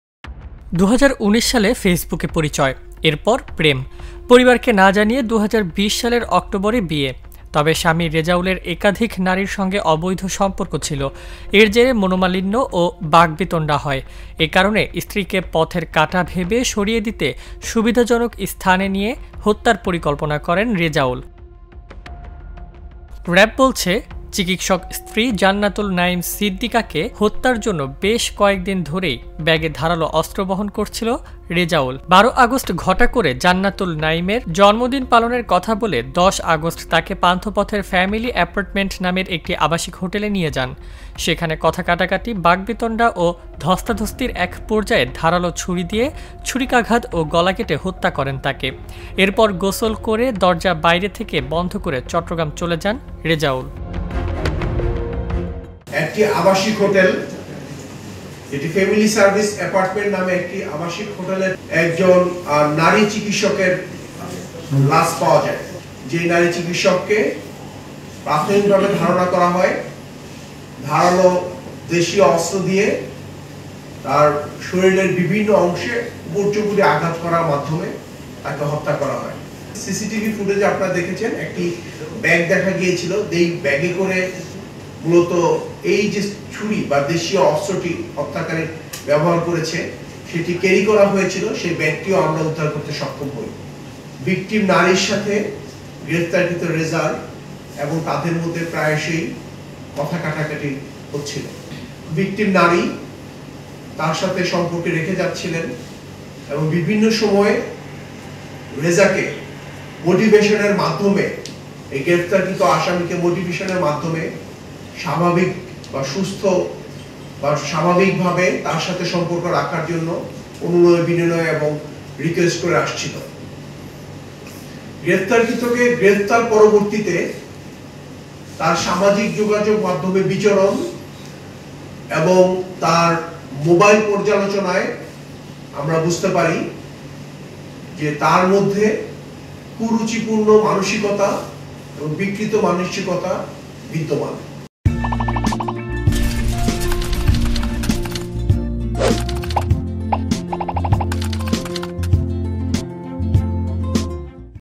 বয়ফ্রেন্ড নয়, ঘাতক রেজা ছিলেন নারী চিকিৎসকের স্বামী: র‌্যাব || Jago News
বয়ফ্রেন্ড নয়, ঘাতক রেজা ছিলেন নারী চিকিৎসকের স্বামী: র‌্যাব